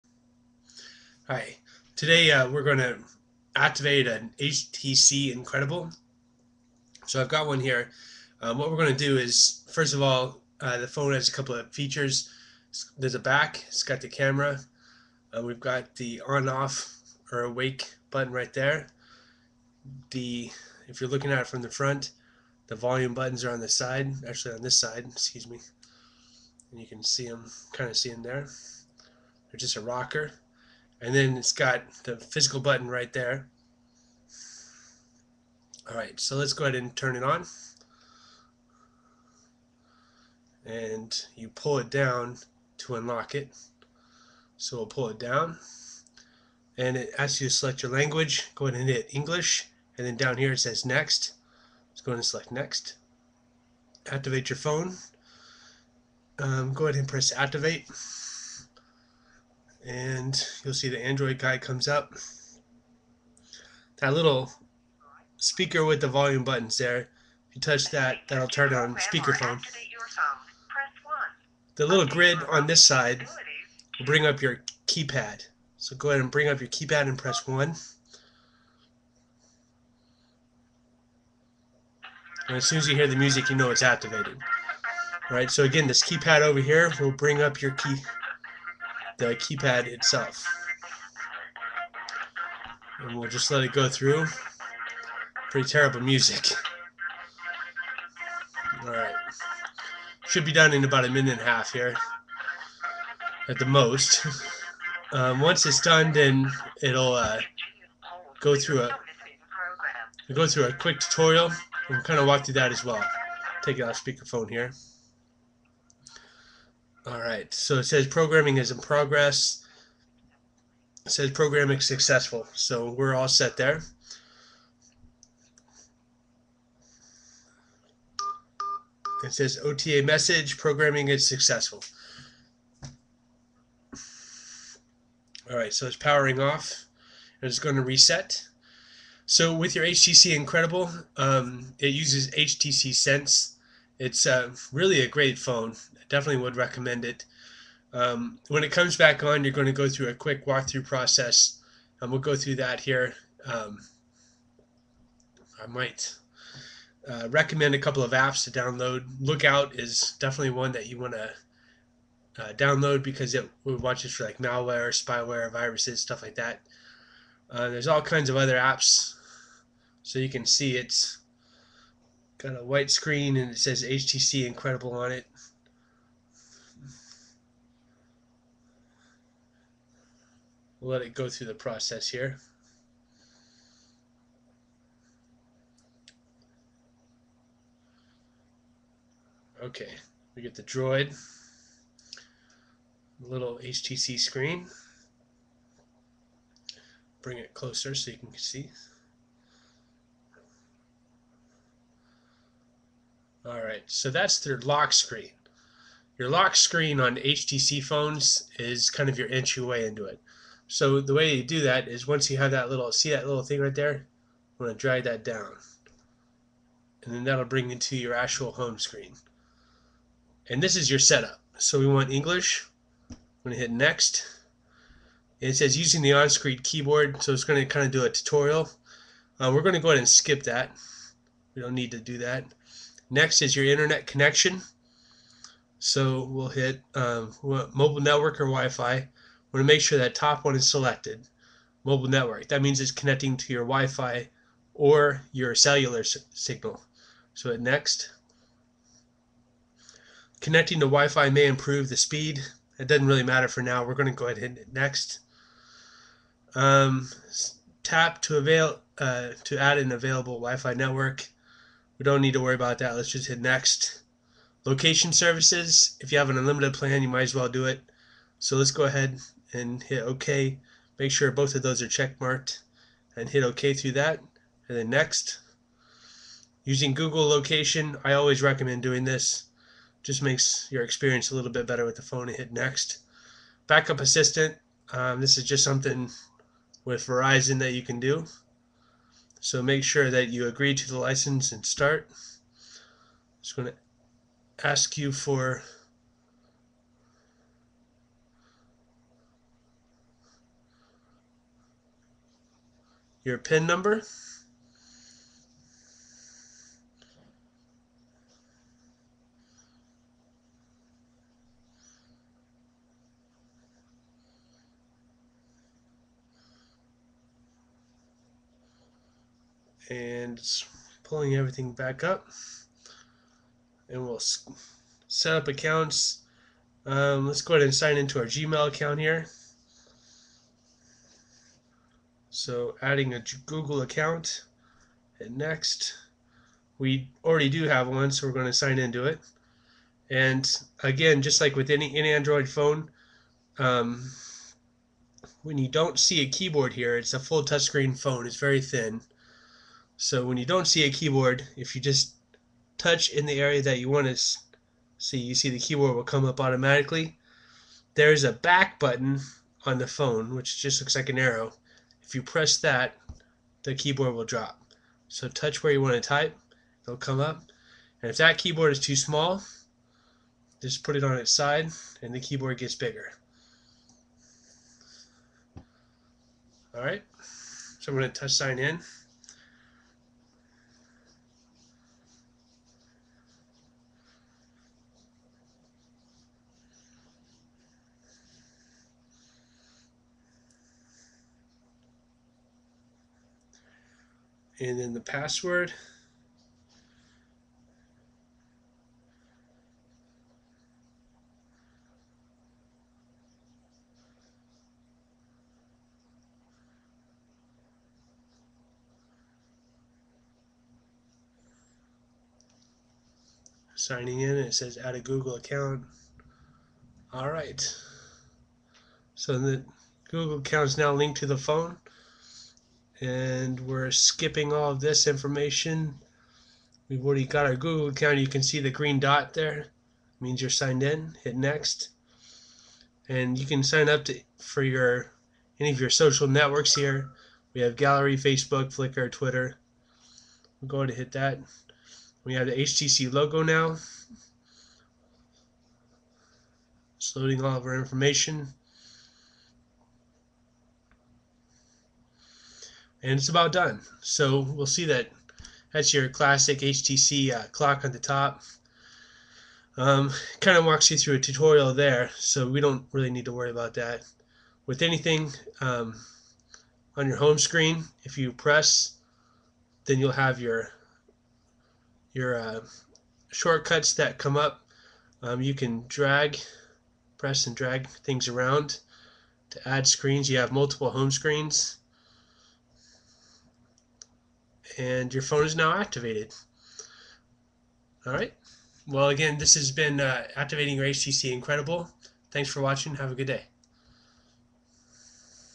How to Activate "Droid Incredible"
Step by Step Walkthrough of Verizon Wireless' Droid Incredible